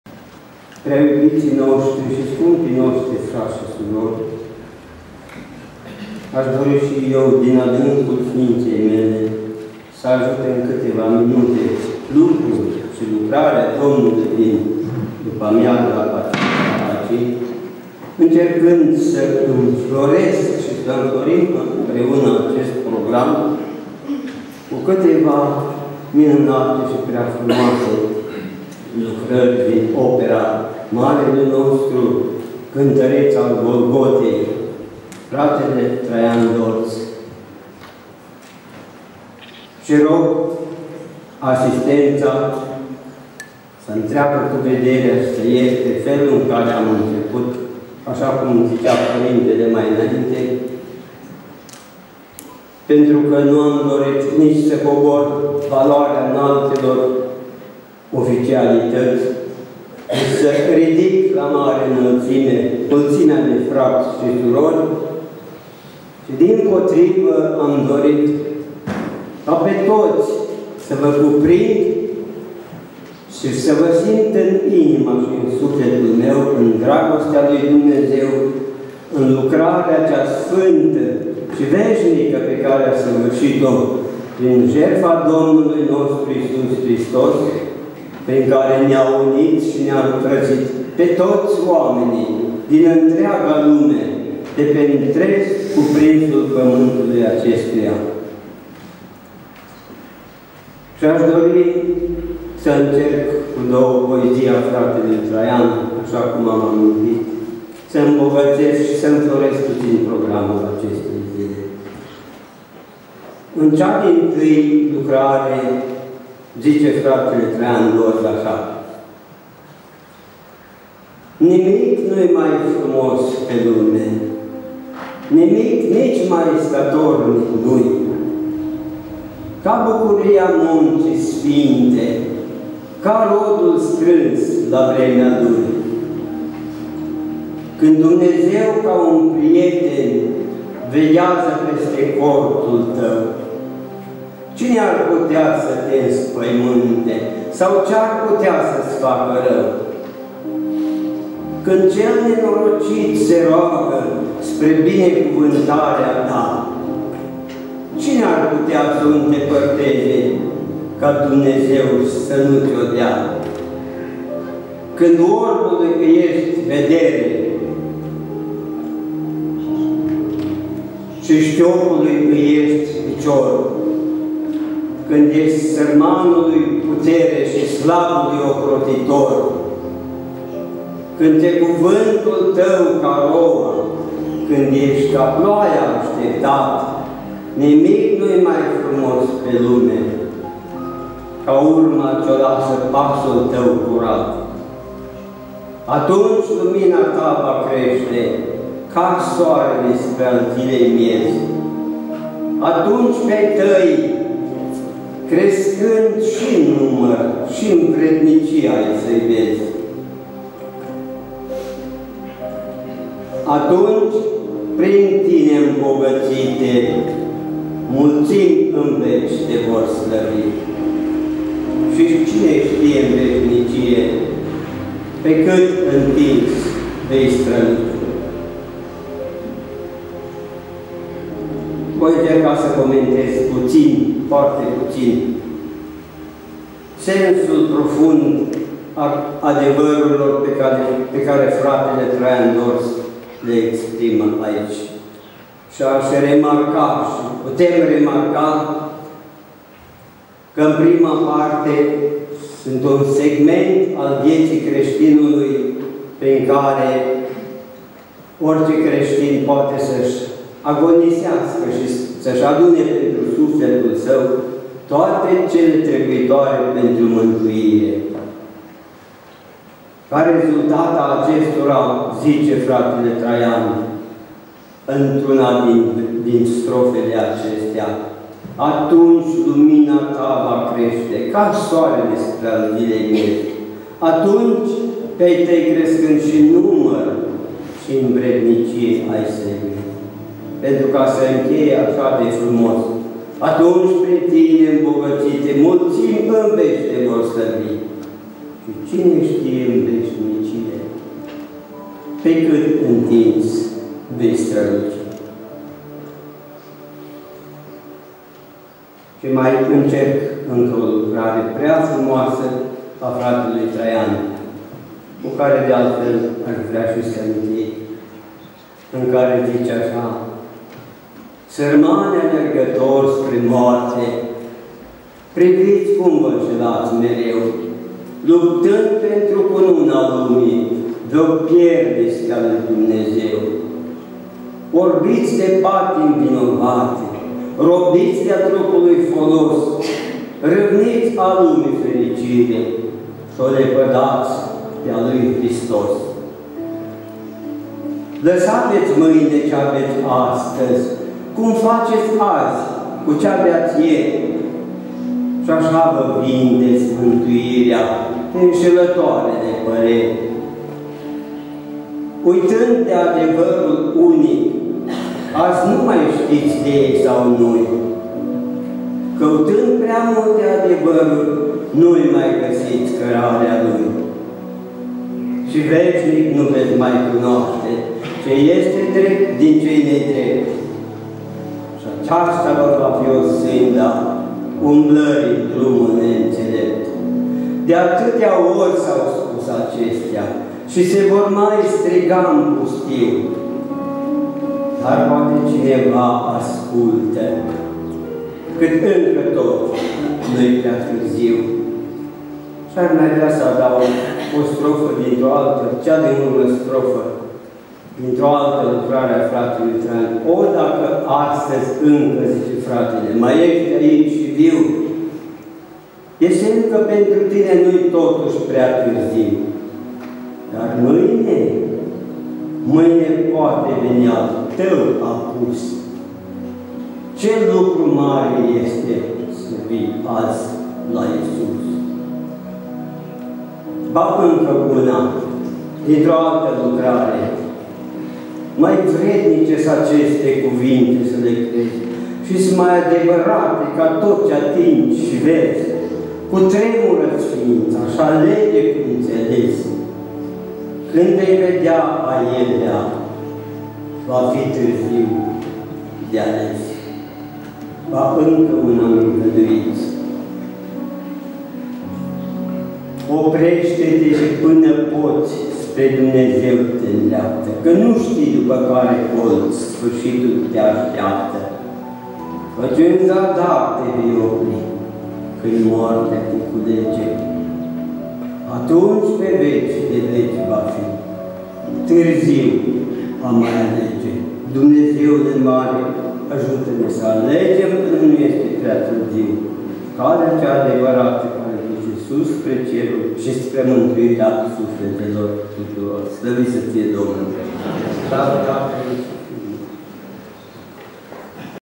Nicolae Vinter - Vorbire (Micherechi, 10 iun. 2012)
Vorbirea fratelui Nicolae Vinter cu prilejul concertului de pricesne organizat de Oastea Domnului şi Bis. Ortodoxă din Micherechi, Ungaria cu participarea tenorului Costel Busuioc (10 iunie 2012)